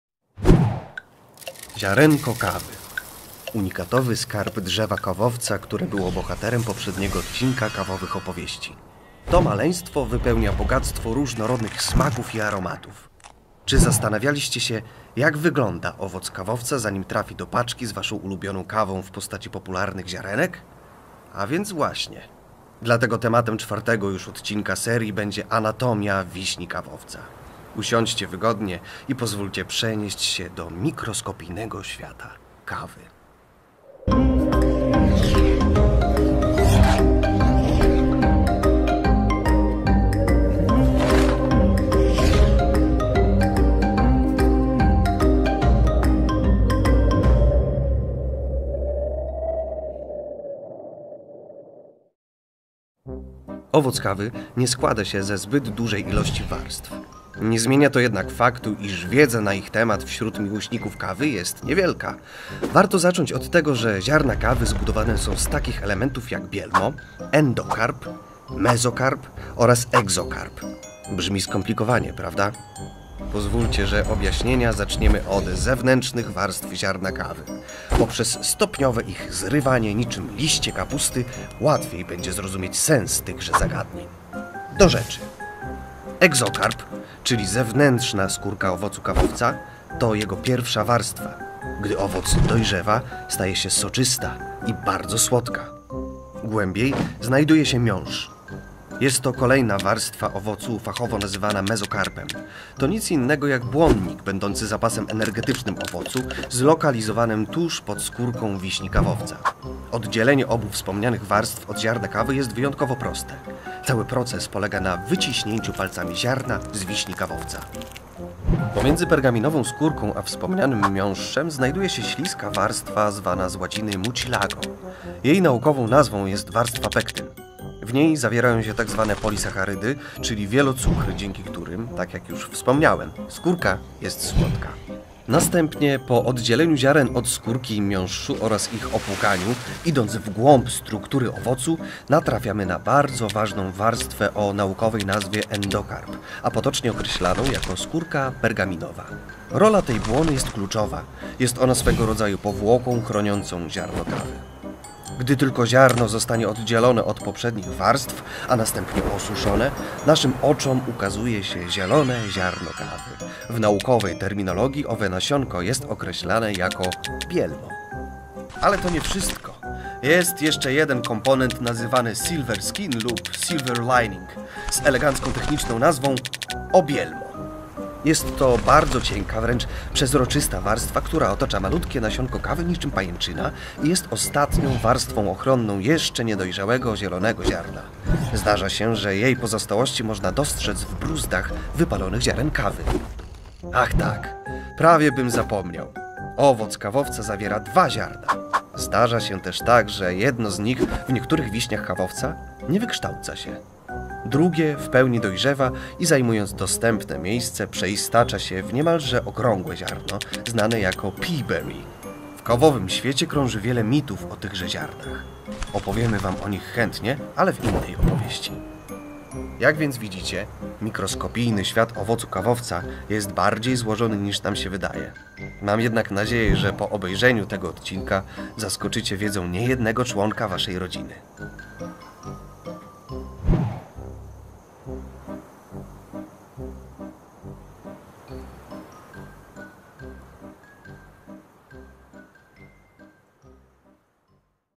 Kawowe Opowieści, ep.4: "Mikroskopijny świat owocu kawowca"
Trochę botanicznej wiedzy o wiśni kawowca podanej w lekkostrawnej formie.